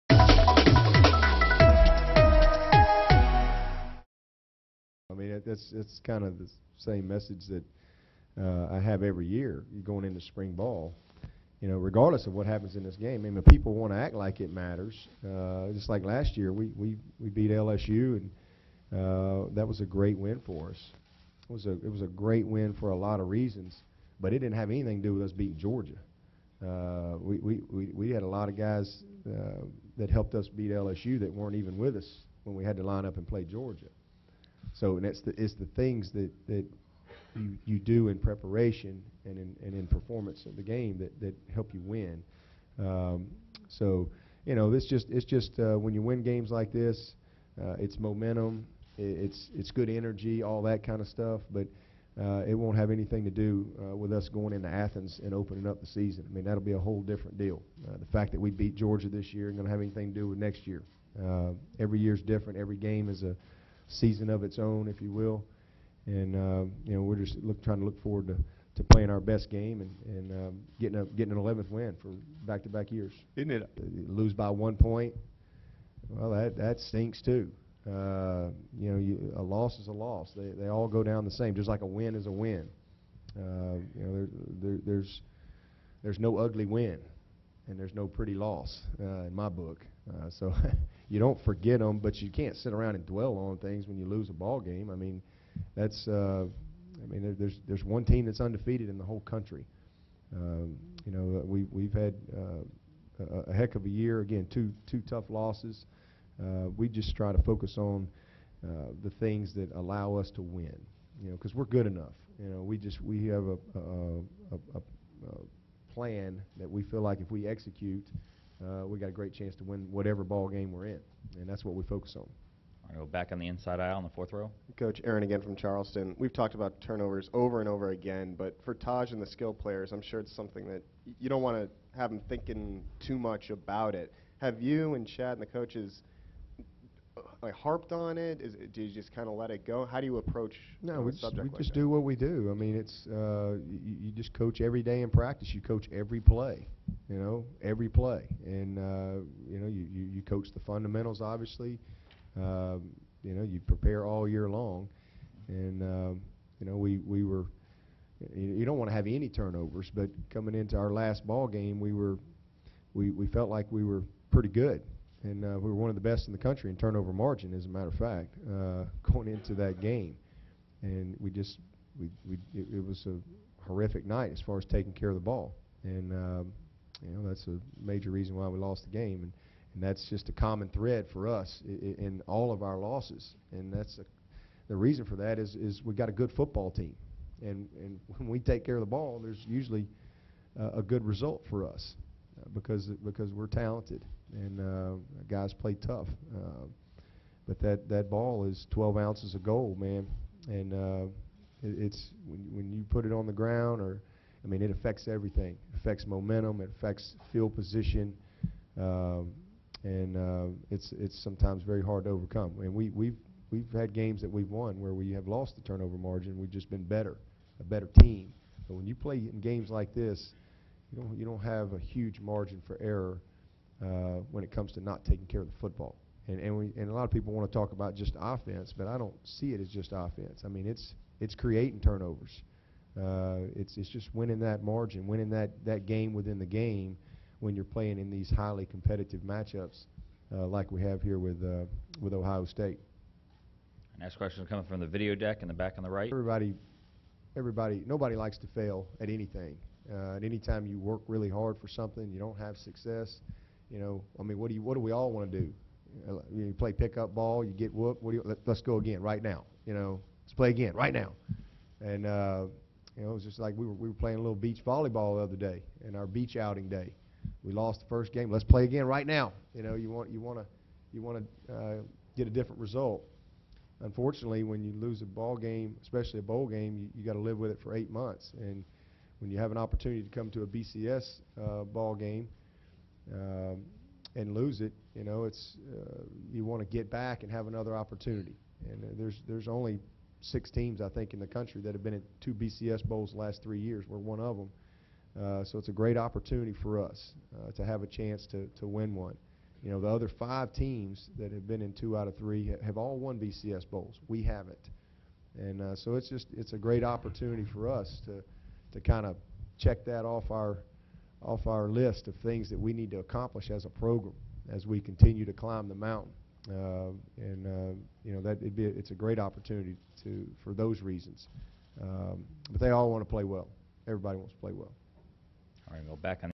Clemson Orange Bowl Presser Part 2
Dabo Swinney discusses the upcoming Orange Bowl